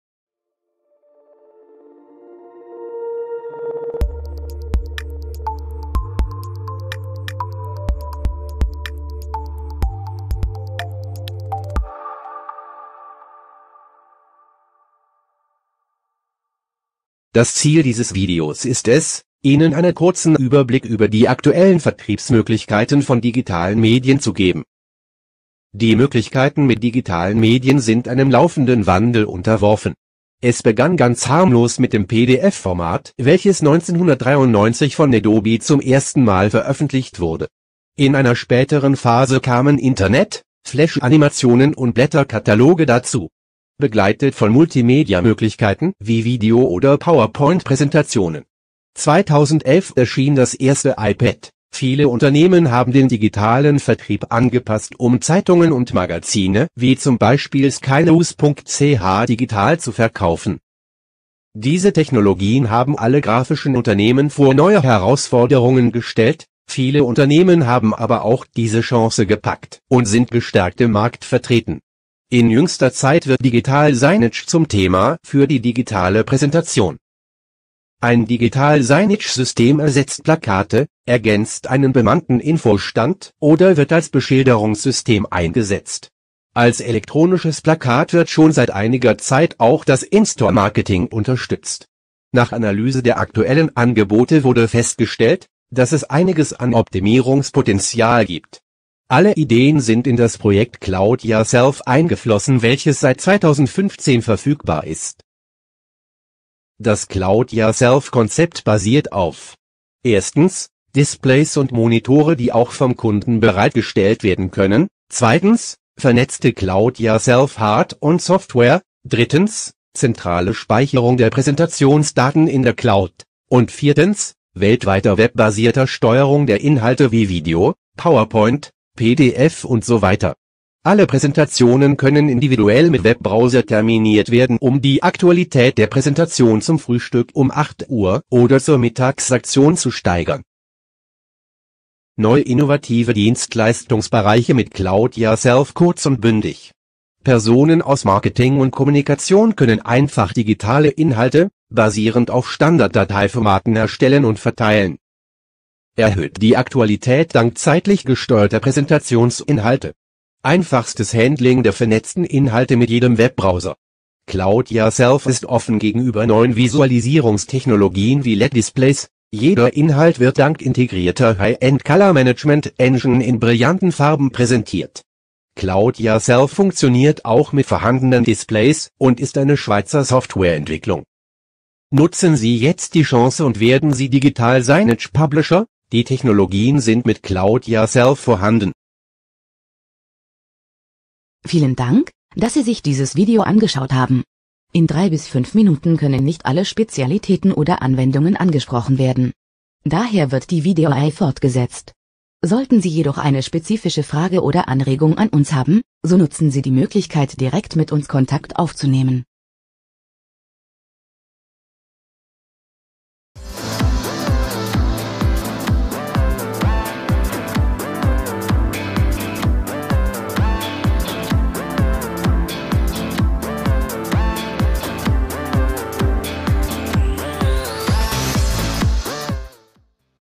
Digitale Mediendistribution – was ist heute möglich?
Was mit PDF begann fand seine Fortsetzung in Internet, Blätterkatalogen, Tablet-Apps und Digital Signage-Anwendungen. Die Digitalisierung gegenüber dem konventionell gedruckten Inhalt bringt neue Chancen und Herausforderungen.
Unser Referent Daniel Scheuber hat mit Hilfe von Daniel Mayerthaler ein Video erstellt, in dem es um Digitale Medien-Distribution im Wandel der Zeit geht.